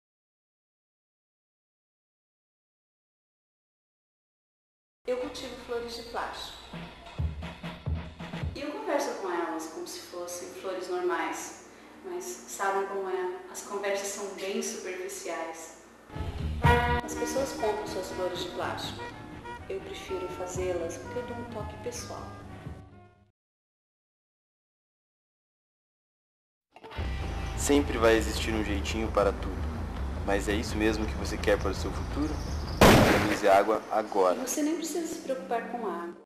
Comercial "Economize Água Agora" - PPTV UFPR
Comercial de 30" produzido em 2006 por alunos da UFPR, para uma campanha em prol da economia de água.